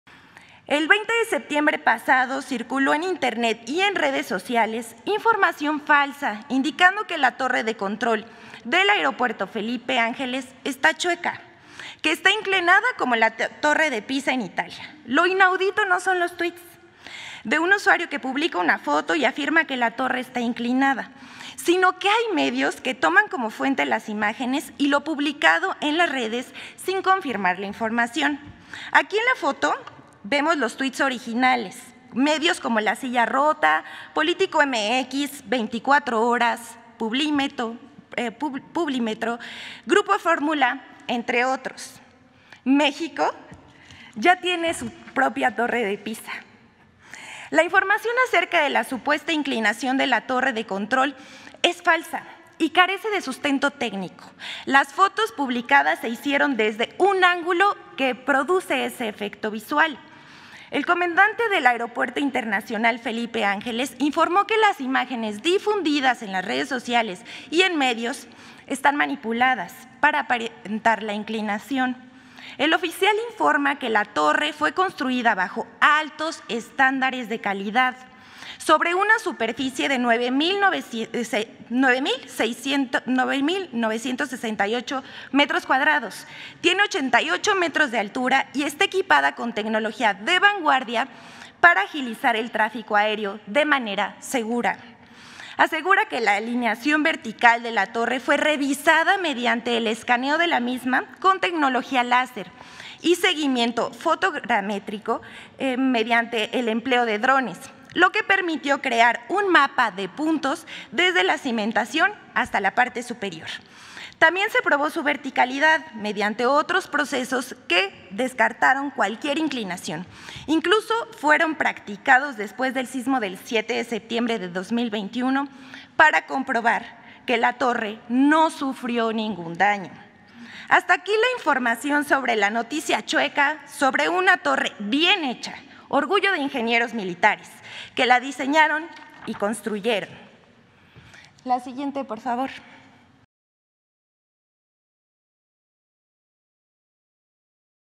Ana García Vilchis asegura que la torre de control del AIFA no está chueca
Ana García Vilchis desmintió que la torre de control de Aeropuerto Felipe Ángeles esté chueca, luego de ser comparada con la Torre de Pisa por una supuesta inclinación en su construcción.